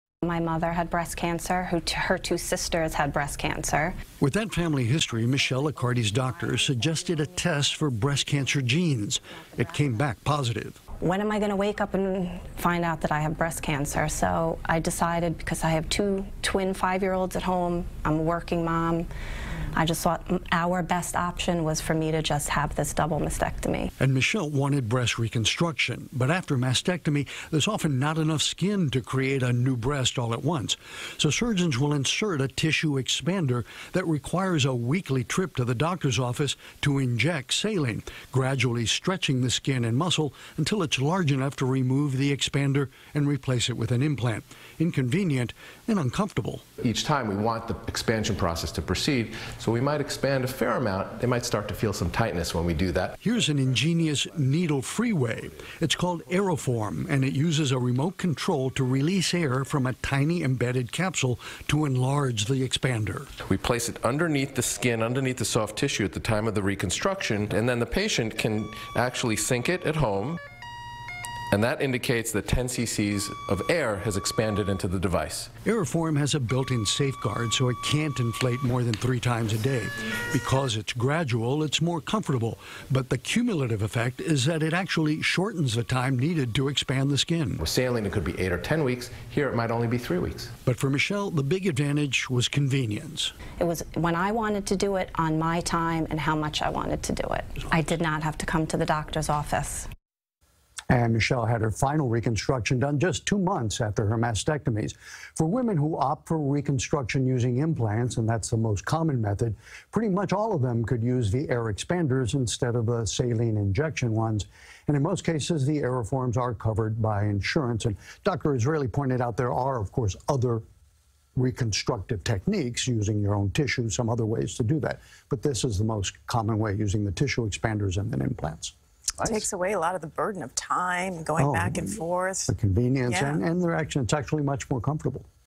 New Procedure Making Breast Reconstruction Surgery More Convenient
It's an important development for the thousands of women who require the procedure each year. Dr. Max Gomez reports.